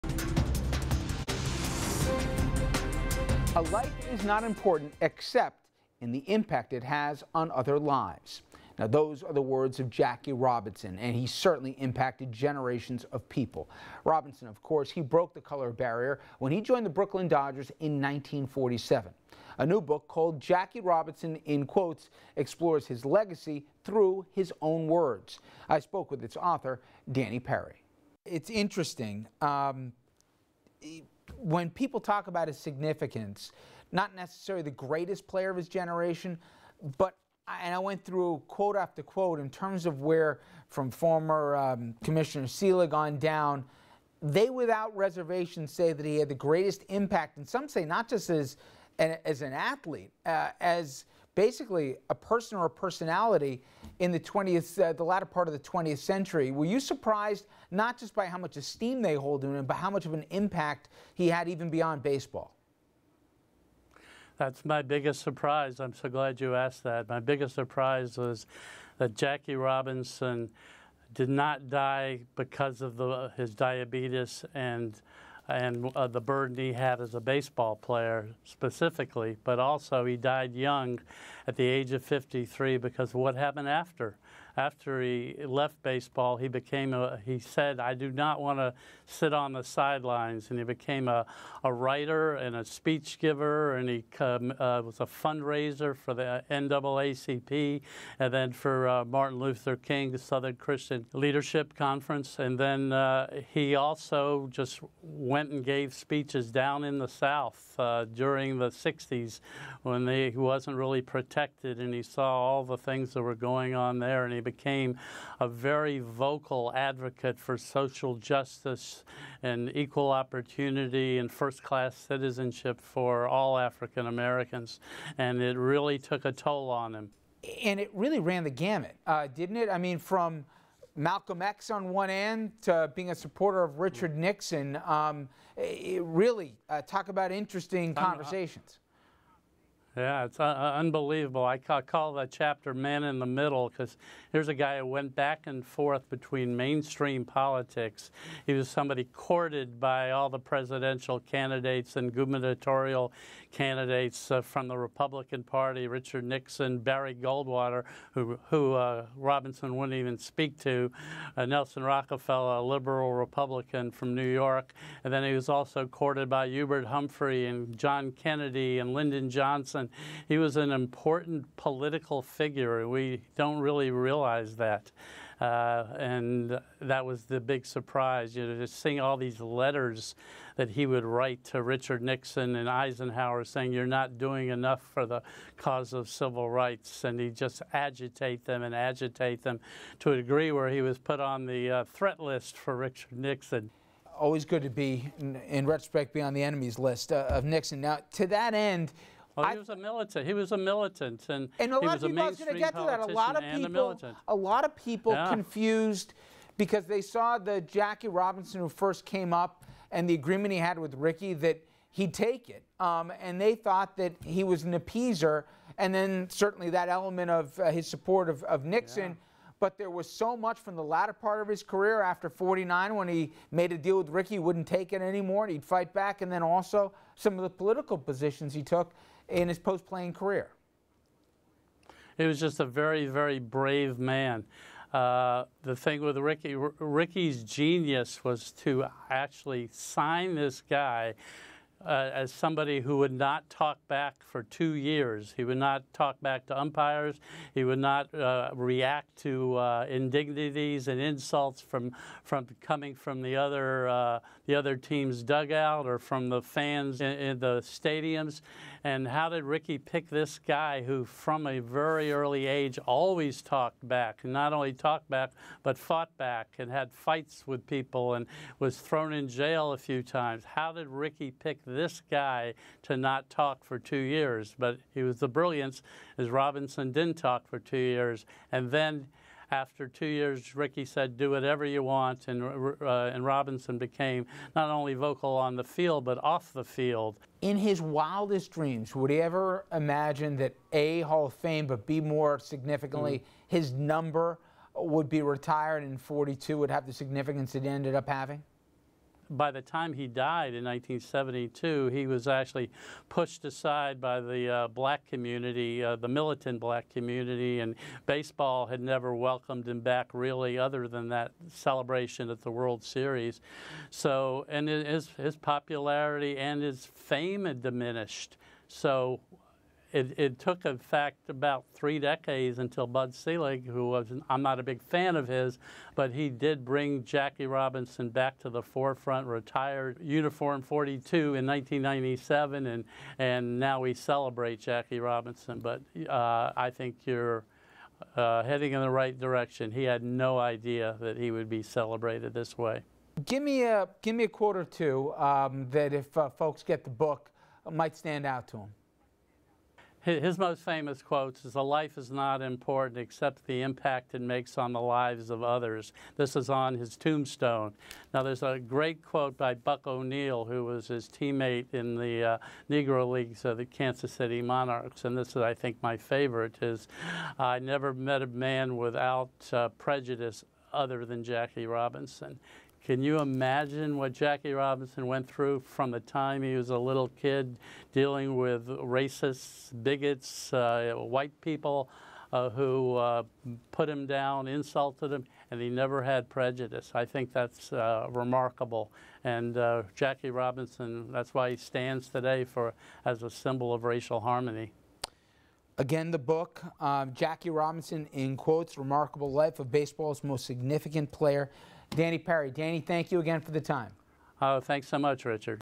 Jackie Robinson In Quotes: New Book About Player Who Broke Color Barrier
RFL host Richard French speaks with Danny Perry, author of a new book about baseball player Jackie Robinson, who broke the baseball color barrier when he joined the Brooklyn Dodgers in 1947.